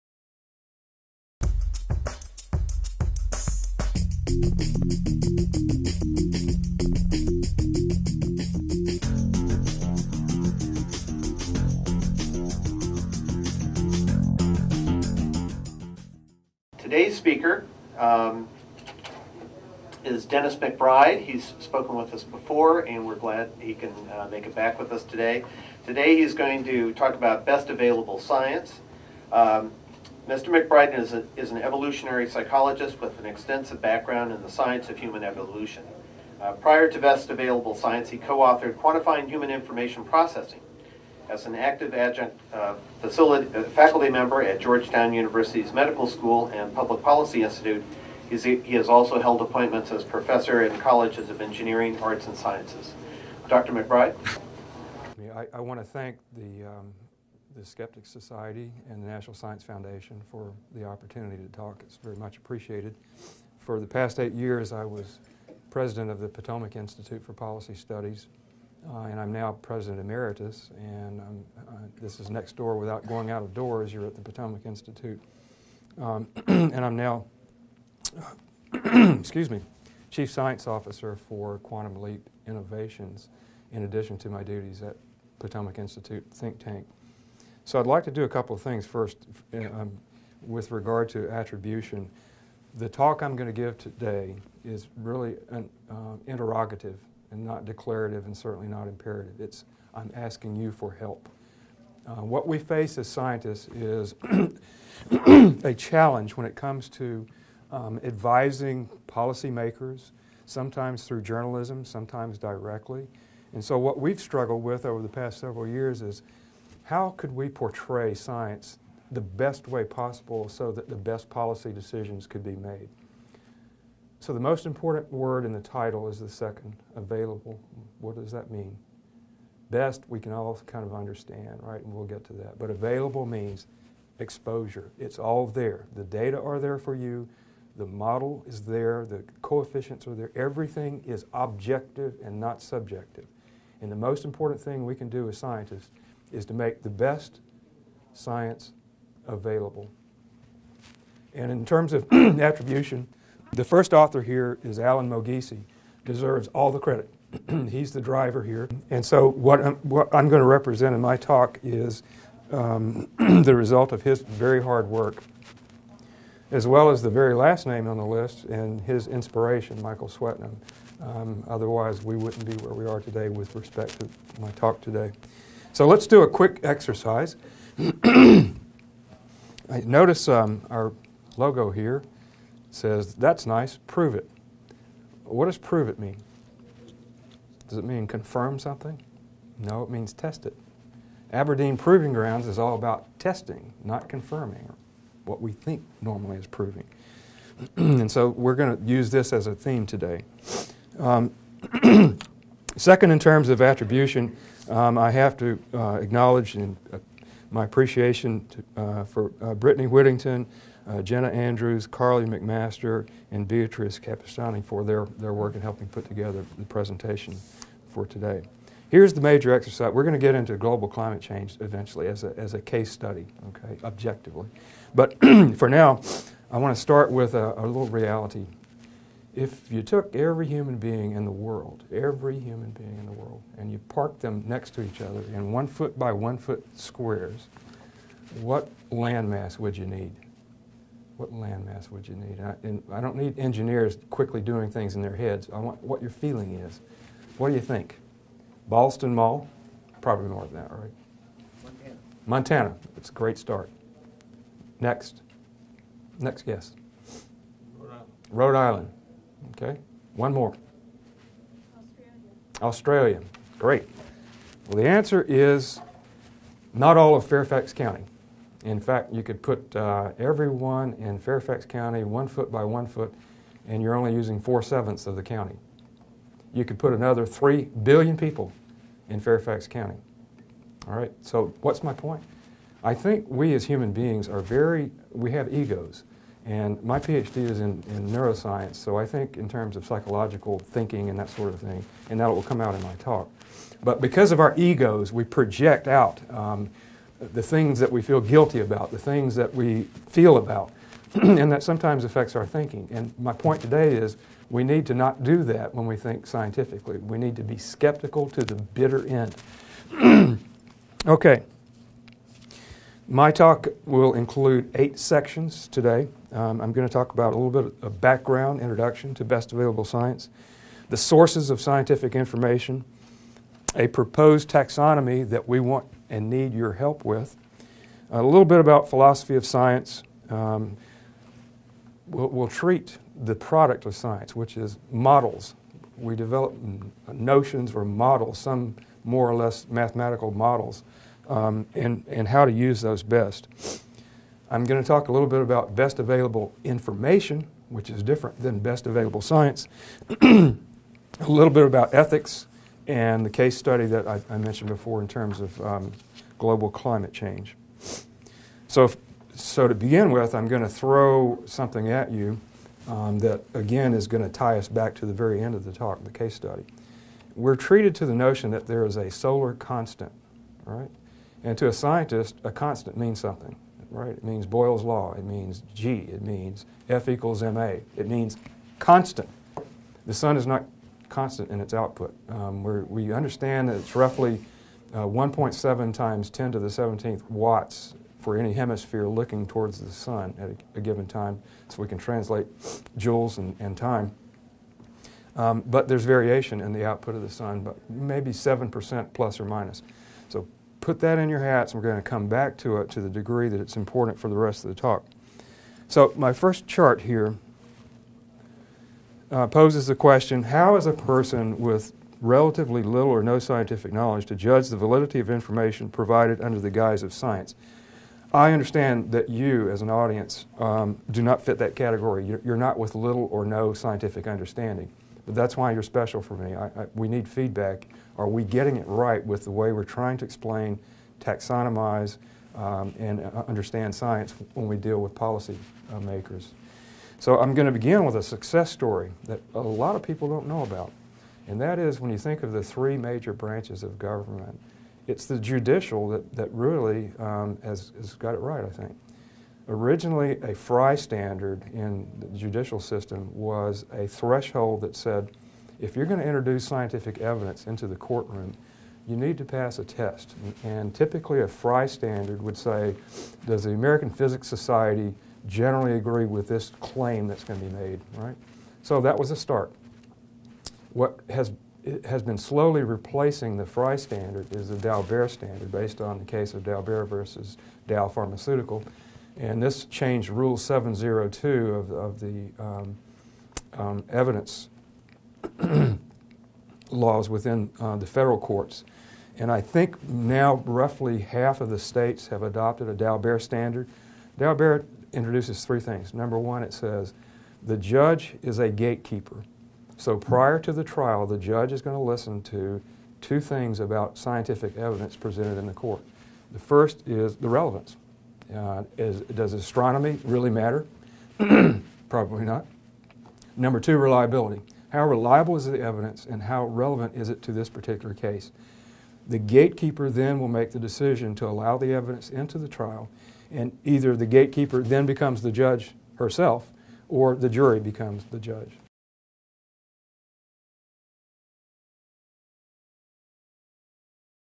1. Best Available Science (Dennis McBride, Ph.D.)
This is part 1 of a 10-part talk

Policymakers, judges, government agency staff, lawmakers, scientists, and others must constantly balance scientific, social, economic, political, emotional and other considerations when making a decision.  However, they are often "on their own" to determine the validity and applicability of scientific and technical data as they ask themselves, "How reliable is the information in front of me? What is the level of maturity of the underlying science, and can I make a sound decision based on it?"

Dennis K. McBride, Ph.D., President Emeritus and Chair of Fellows at  the Potomac Institute for Policy Studies, will discuss the book, "Best Available Science," that he co-authored with researchers from Potomac and the Institute for Regulatory Science. Led by the lead author, Alan Moghissi, this book is the culmination of three decades of insight from experts in the scientific and policy worlds, who have developed a process to enable not only scientists, but also non-scientists, to clearly and easily assess the maturity of scientific information. The book isolates scientific issues from other issues at play in policymaking.  It provides a framework for understanding the validity of scientific information that can be universally understood.  It gives decision makers a simple assessment system to most effectively utilize scientific information.  It also separates science from topics that are outside of the purview of science. As we continue to strive for excellence in both science and policy in an ever-changing landscape, Best Available Science, is a compelling resource.  It arms you with the ability to make the best decision possible with the best available information.  This guide can also be used as a tool for scientists to more clearly communicate with policy makers and help them to "speak the same language."

Dennis K. McBride, Ph.D. is President Emeritus and Chair of Fellows at the Potomac Institute for Policy Studies, a Washington area think tank dedicated to the provision of science and technology policy expertise to the administration, congress, and the judiciary. Members of the Institute remain actively engaged in the scientific disciplines for which they provide expertise. McBride is an evolutionary psychologist with an extensive background in the science of human evolution. Prior to Best Available Science, he co-authored "Quantifying Human Information Processing" (Rowman & Littlefield).

McBride is Chief Scientific Officer for Quantum Leap Innovations.  In addition to being an active adjunct professor at Georgetown Universitys medical school and Public Policy Institute, he has also held appointments as professor in colleges of engineering and arts and sciences.

Views expressed are those of the speaker and do not necessarily represent the views of the National Capital Area Skeptics.